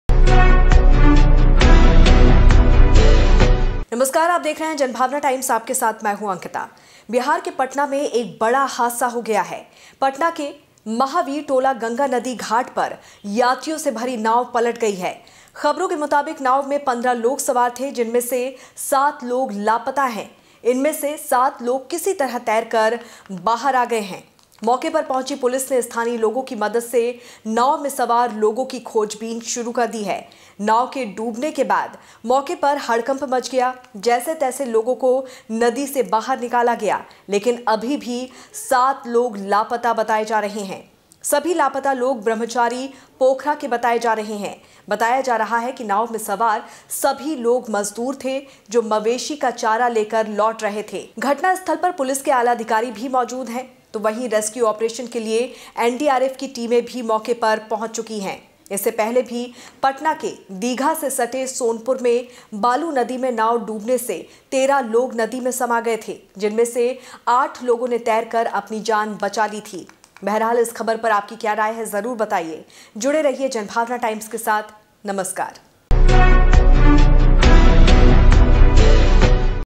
Bihar News: बिहार में गंगा नदी में नाव पलटने से 7 लोग लापता | Latest News | JBT News
बिहार के पटना में एक बड़ा हादसा हो गया। पटना के मंदिर महावीर टोला गंगा नदी घाट पर यात्रियों से भरी नाव पलट गई। खबरों के मुताबिक नाव में 15 लोग सवार थे जिनमें से 7 लोग लापता है।

A major accident took place in Patna, Bihar. A boat full of passengers overturned at the temple Mahavir Tola Ganga river ghat in Patna. According to reports, there were 15 people in the boat out of which 7 people are missing.